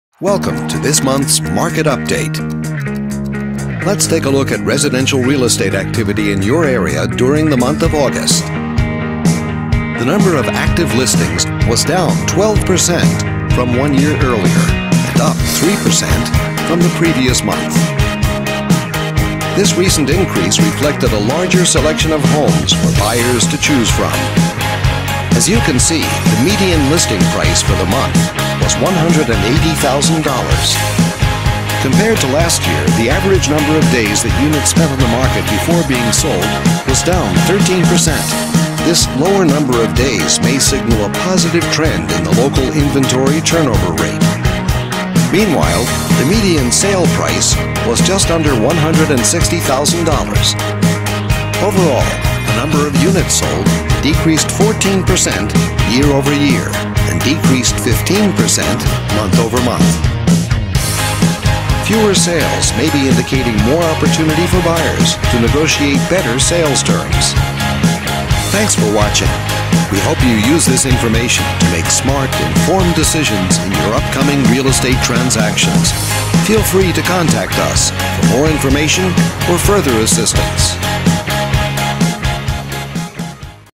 Davenport Market Update,FL, Real Estate Market Update from Watson Realty,August,2013
Watson Realty Video Update recapping market activity within Davenport Market Update, FL, during the month of August, 2013